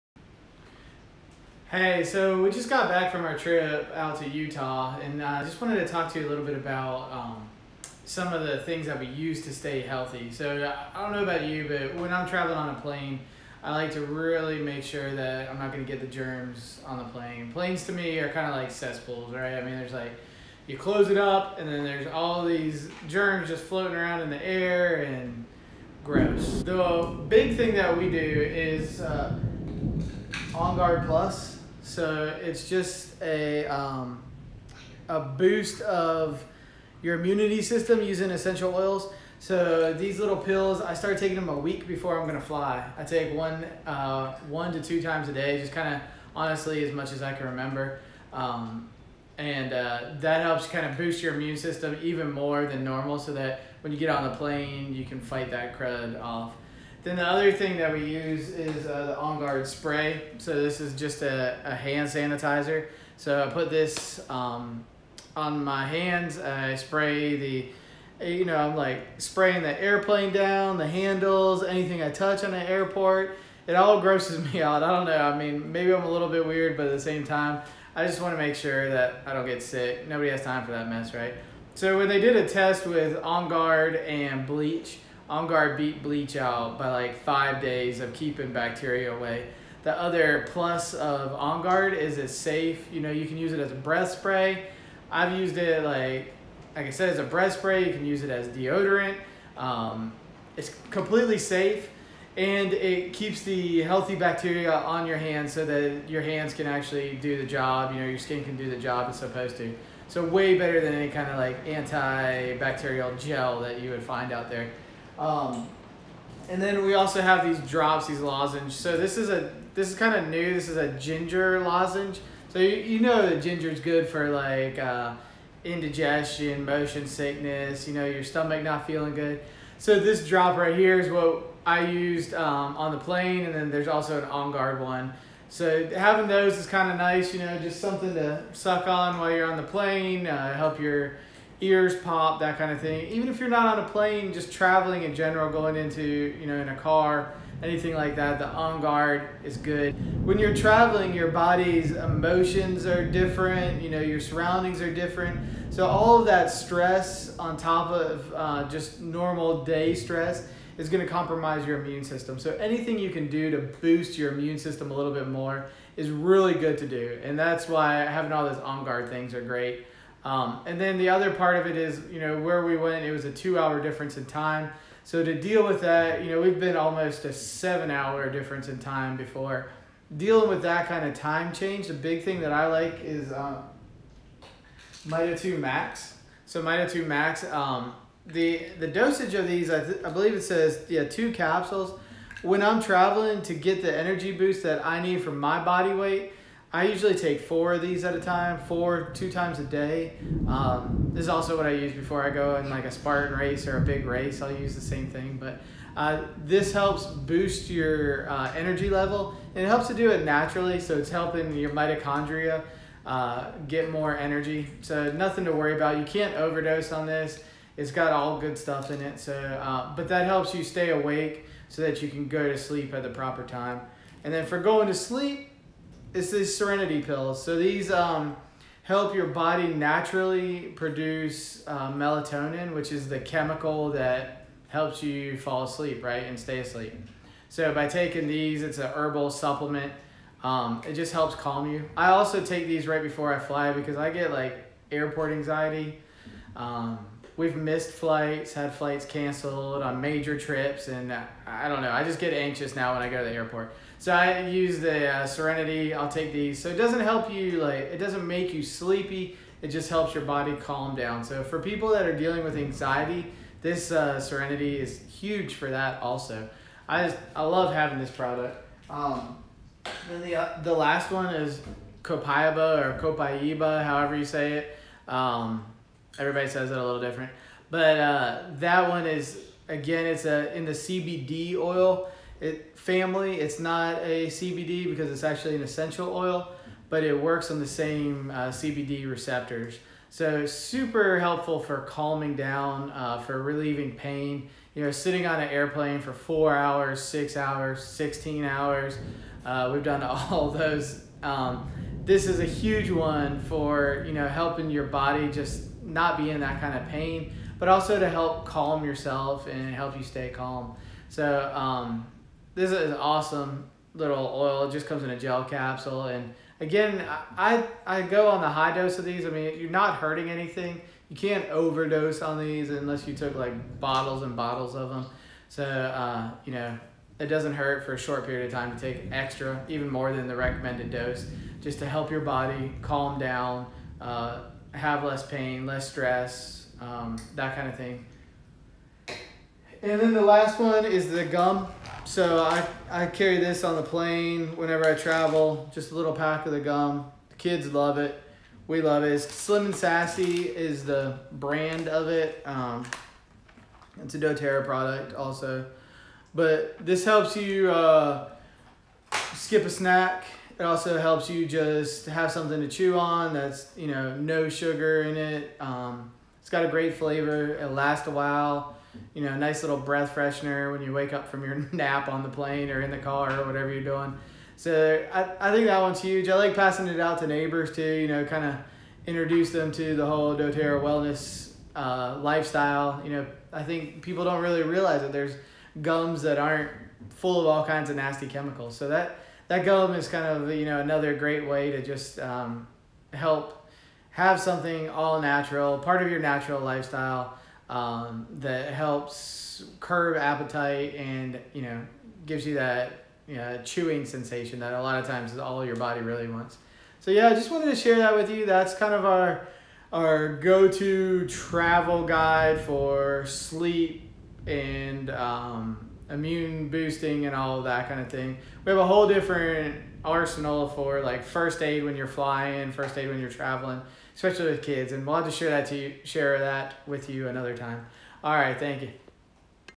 The best ways to stay healthy while traveling.
Traveling makes you sick!

You get in a car or on a plane and you got out of your element. You don't eat the same foods. Usually, people eat more sugar and carbs, drink a little bit more and have an extra layer of stress from not being in their surroundings.

Does all of that sound familiar to you?

We just got back from a trip out to Utah and it was all true. I don't really like to travel that much. I like to be in new places, see new sights, eat local food, and of course try new coffees! BUT...the act of getting there is not my favorite. I don't like that chance of missing a flight, missing a turn, or all the germs that come with airplanes and airports.

Luckily we now have a huge assortment of natural products that help keep all of the above in check. Supplements to help with dealing with the time change. Other supplements to help with sleeping, even essential oils that help with boosting our immune system and cleaning surfaces of germs.

Having all of those tools has for sure made traveling so much better!